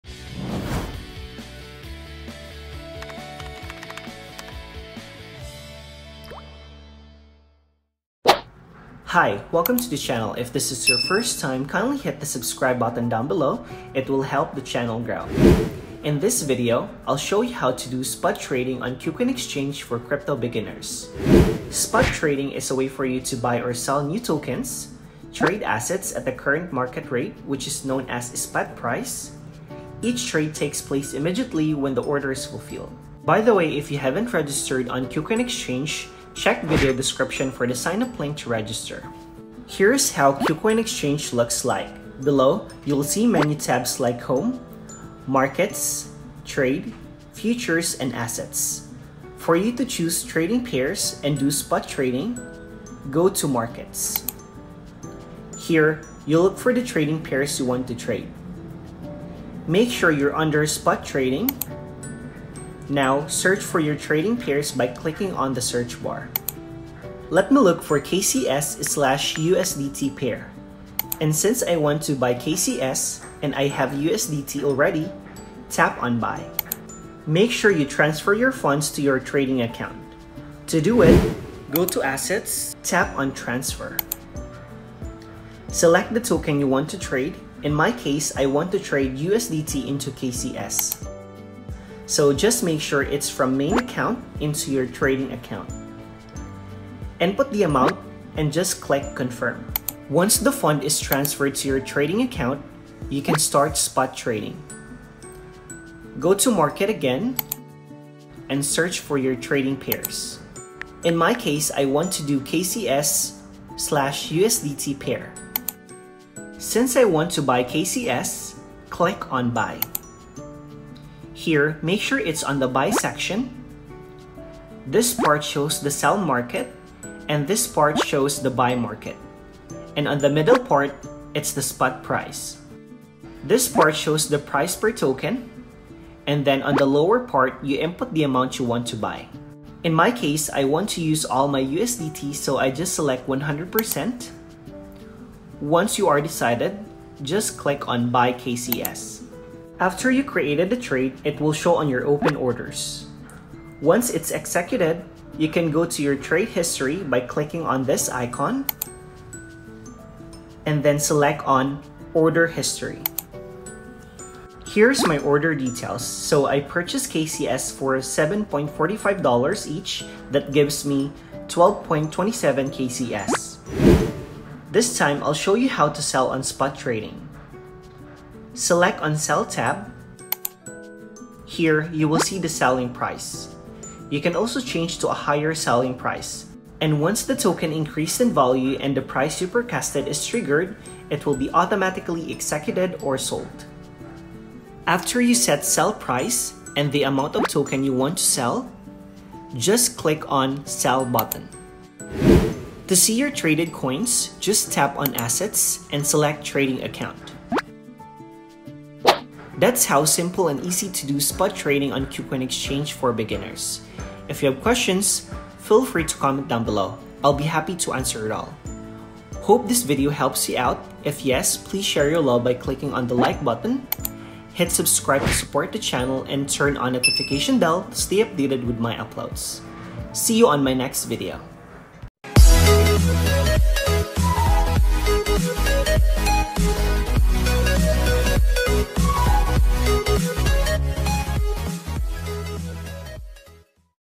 How to TRADE on KUCOIN Exchange mobile app for BEGINNERS | Spot Trading Tutorial
Here’s a video tutorial on how to do Spot Trading on Kucoin Exchange for beginners. Spot trading is a way for you to trade and BUY/SELL tokens on the crypto market. Check video to learn more!